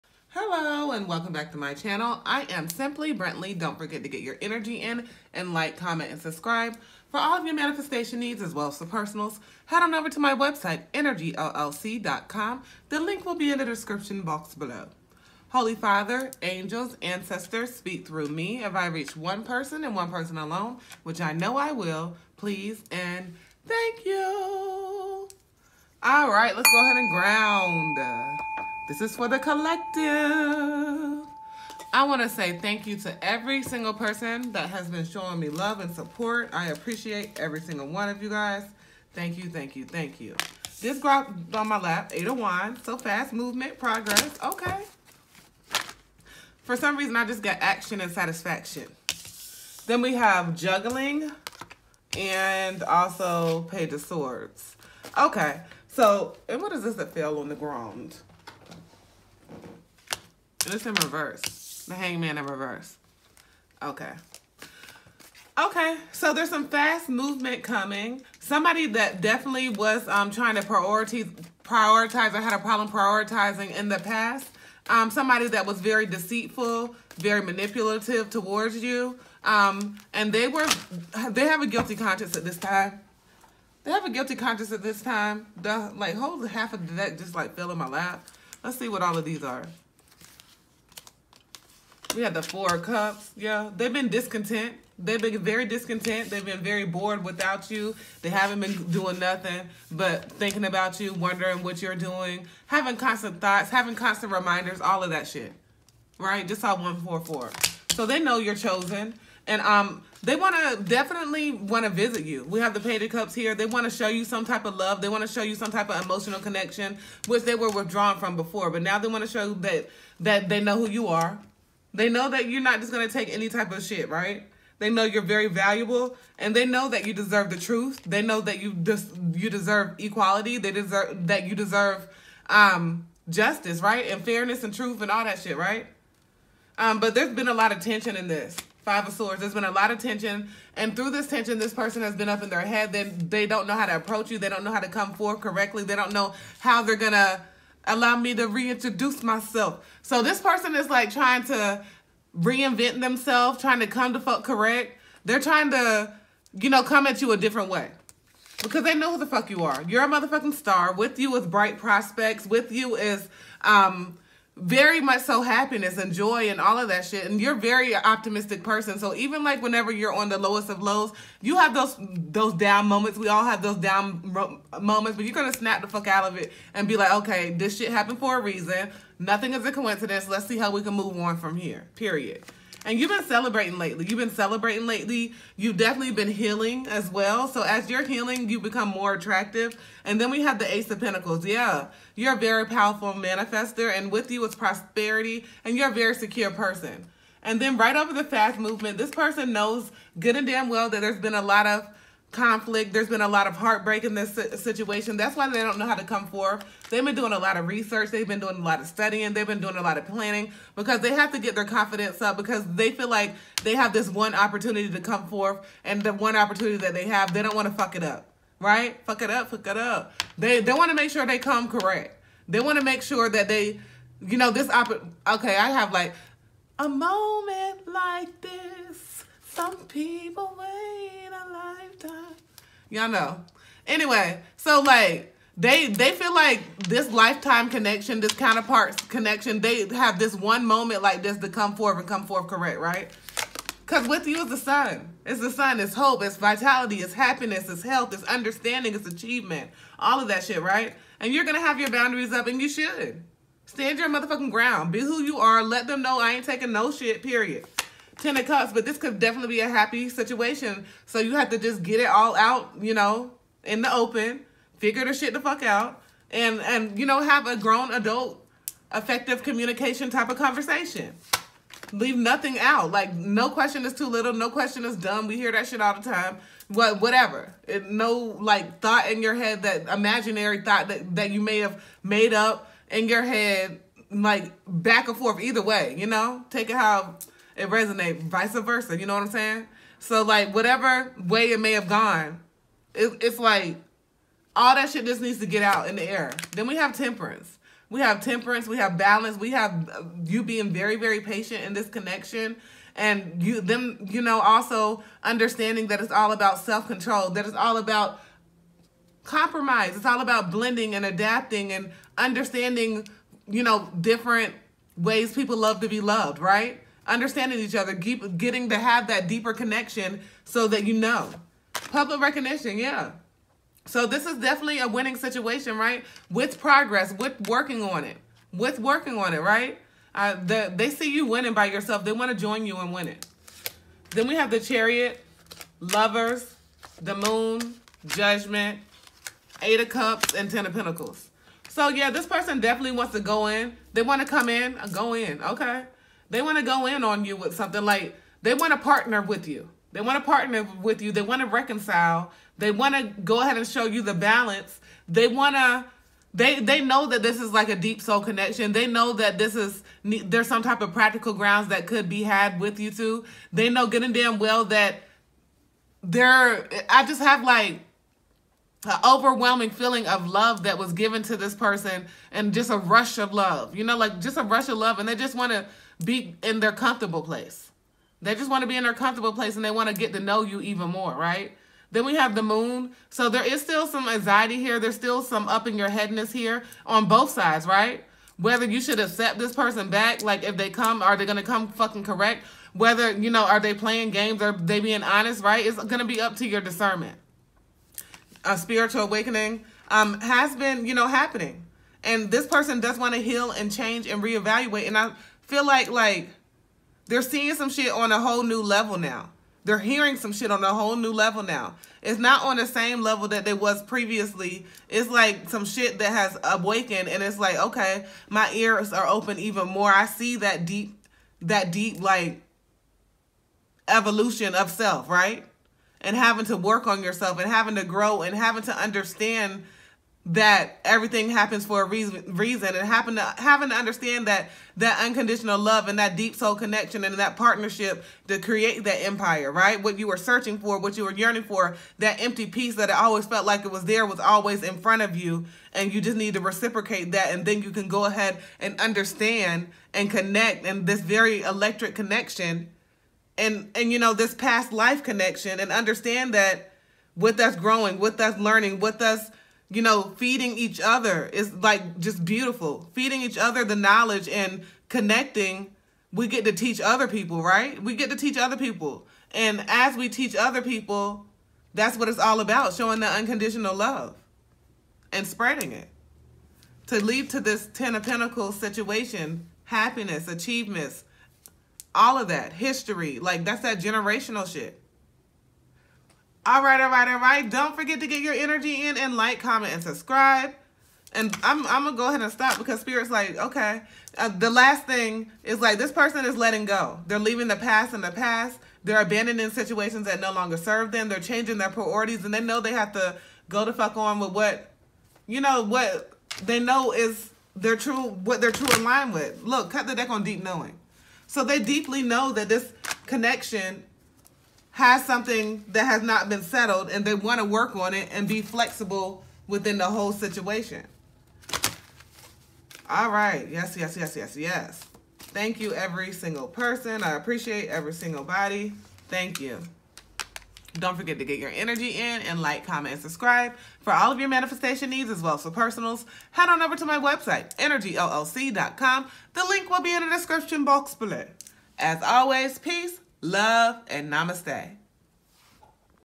🧿🦋🔮THIS PERSON HAS A UNDENIABLE PASSION FOR YOU!!!🦋😘❣️
Let’s see what messages SPRIT has for us today!!! As always take what resonates and leave the rest as it is somebody’s message💕❤️😍❤️
💕Facebook 💕Innergeellc
      💕💕Peace, Love & Namaste 💕💕